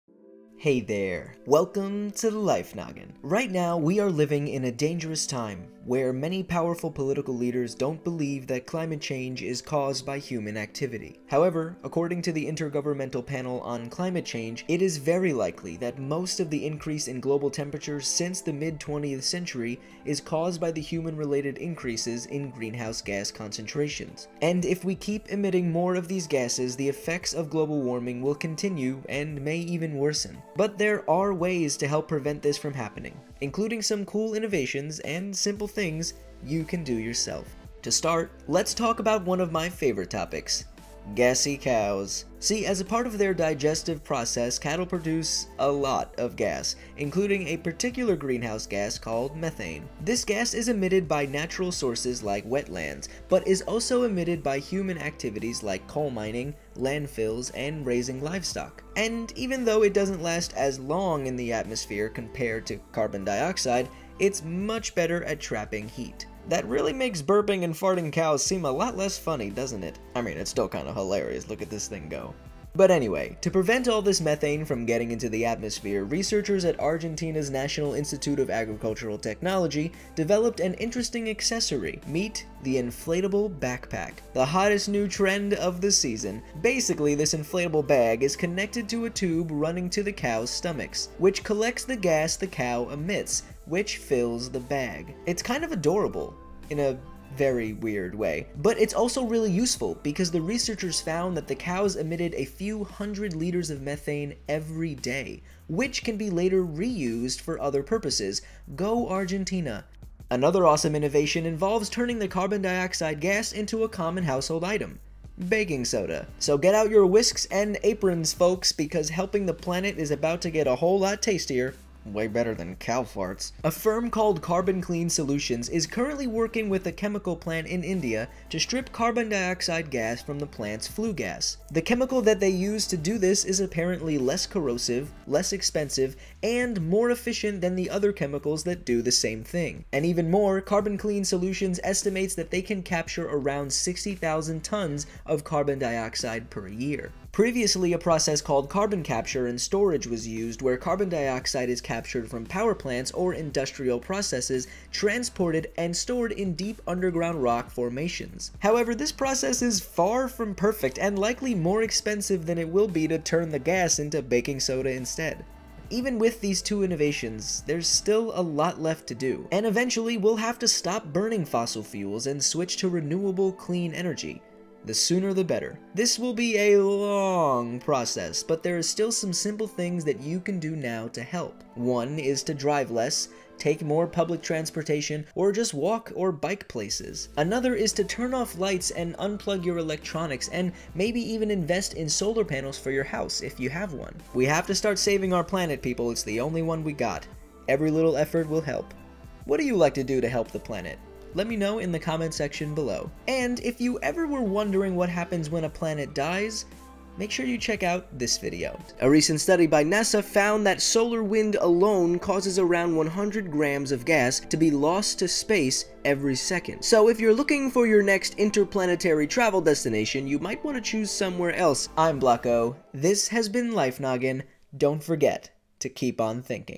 Can We Reverse The Damage Done To Earth?
Man-made climate change might be ruining the world around you, but luckily, scientists are working on several solutions.